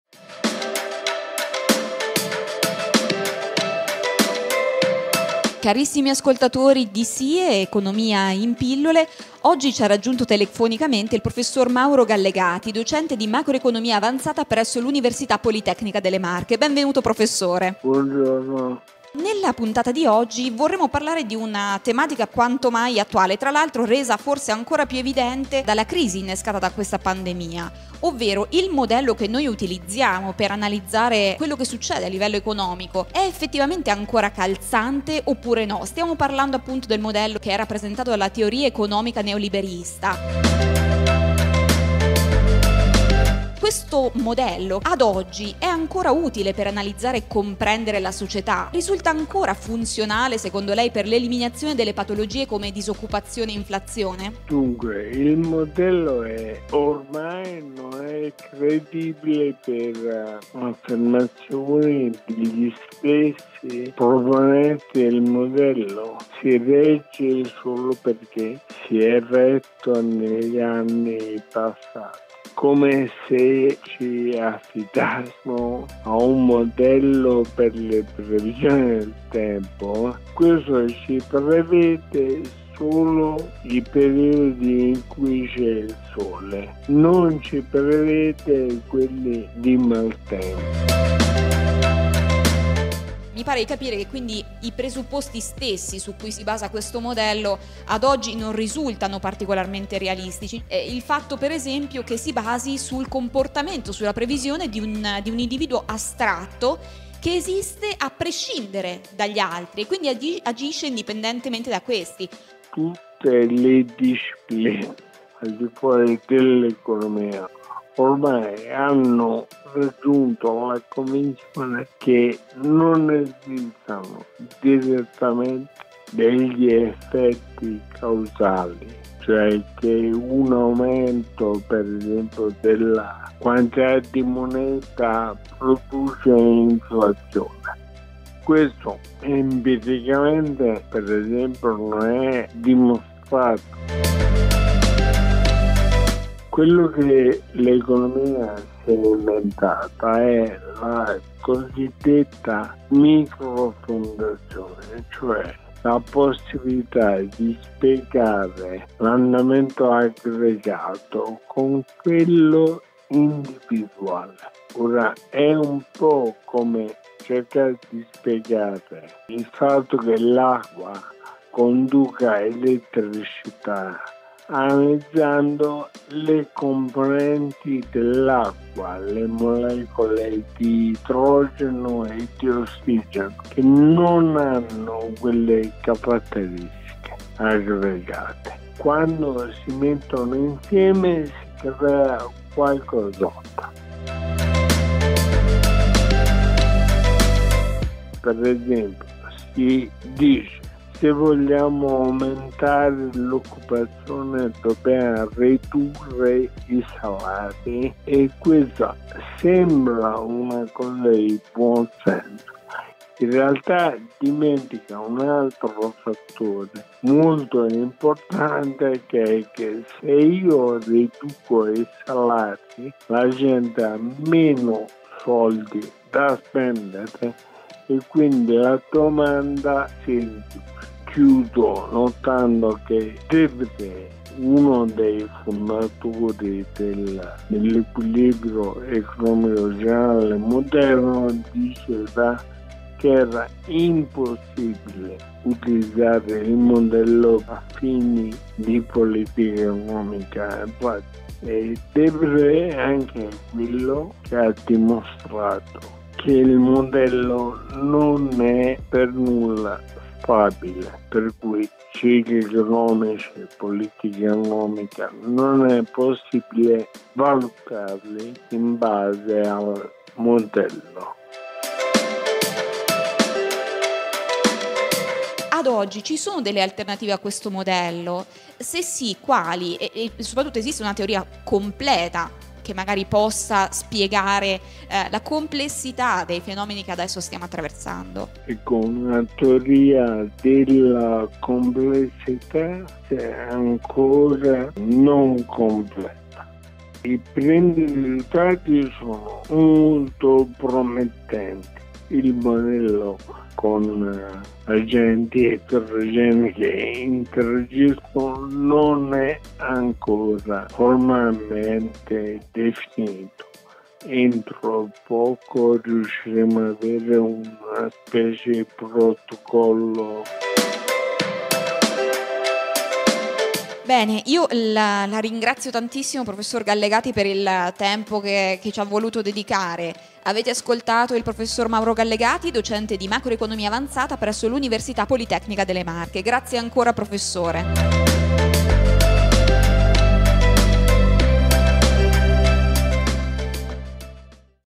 14) Crisi della teoria economica neoliberista: i limiti del modello mainstream
"Crisi della teoria economica neoliberista: i limiti del modello mainstream"
Prof. Mauro Gallegati (Università Politecnica delle Marche)

SIE - Economia in pillole
Ep.N.14, dell'11 gennaio 2021